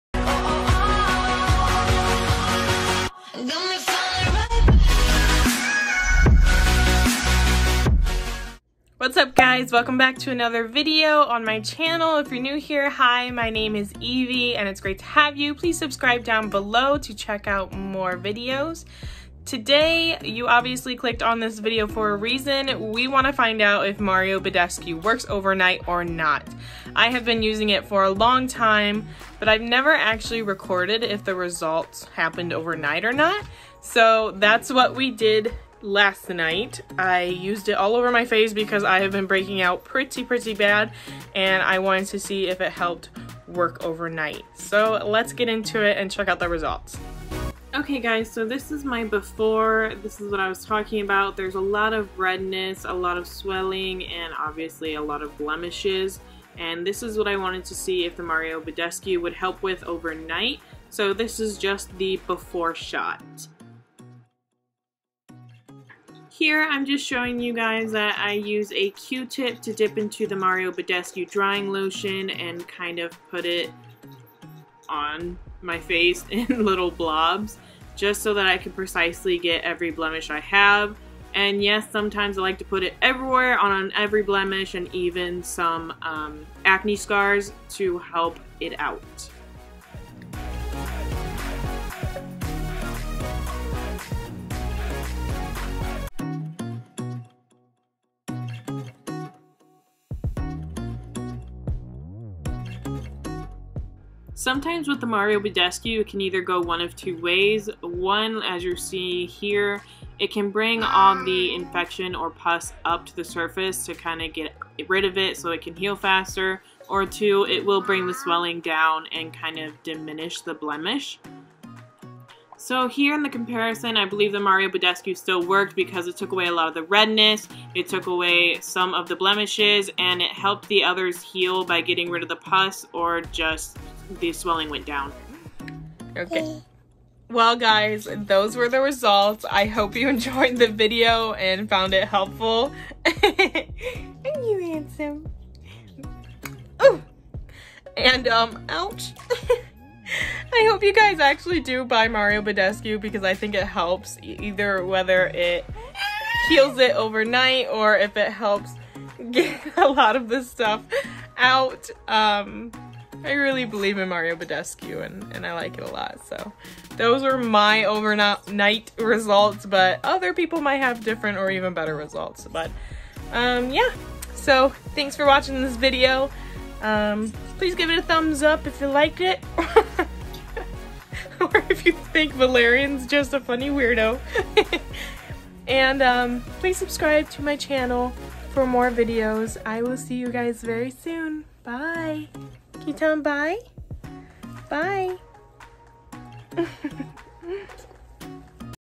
DOES MARIO BADESCU WORK OVERNIGHT? *RESULTS*
Hey guys! today I am showing my results from using Mario Badescu for only one night. enjoy!